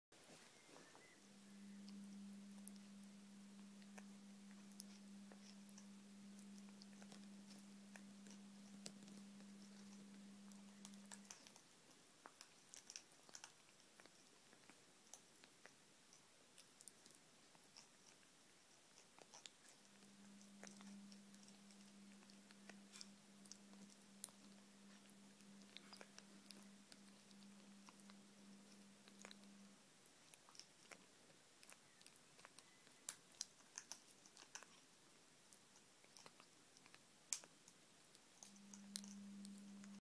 Kip geniet van zijn fruitcupje.
Grijze Roodstaart Kip smult hoorbaar van zijn fruitcupje.
African Grey Kip eating his fruitcup.